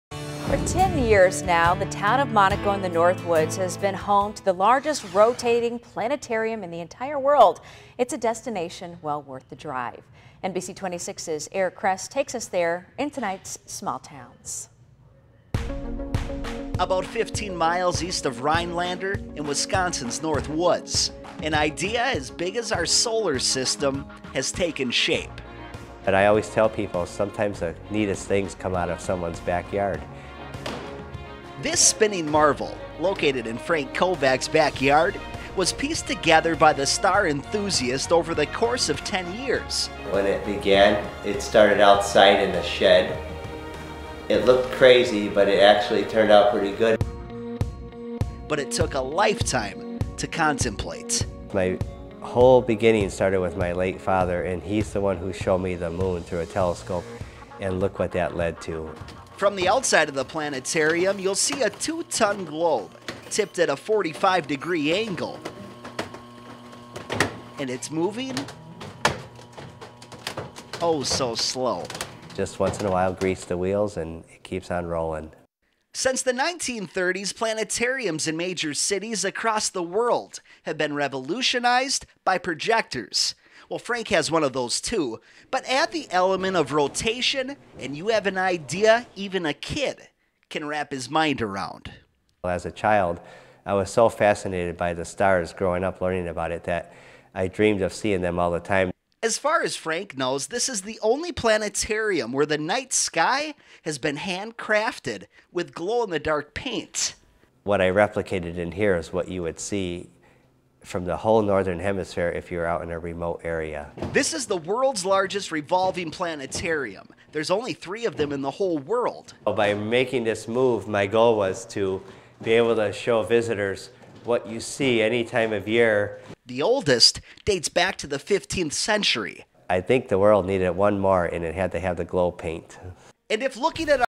Small Towns: Kovac Planetarium
It's a truly one of a kind experience to see a planetarium that was hand made and designed by a dreamer in the North Woods of Wisconsin. The structure took Frank Kovac ten years to fully complete but for those who check it out, it's fully worth all of the effort.